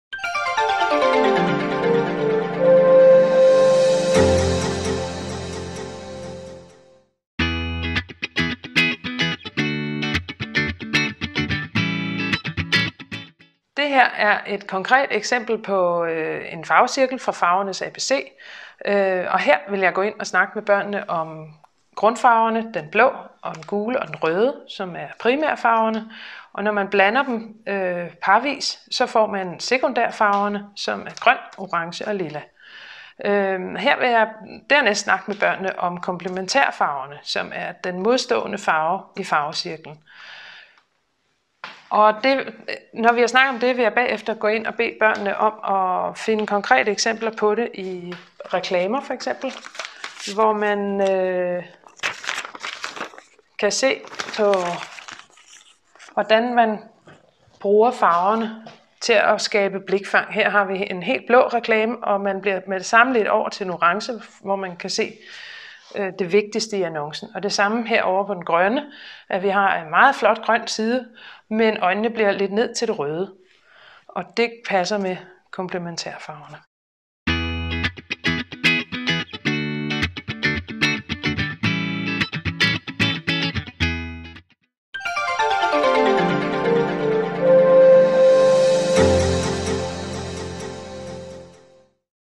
Farvernes ABC - undervis i komplementærfarver
Lisbet fortæller om komplementærfarver, hvor hun kommer med eksempler på undervisning i farvelære.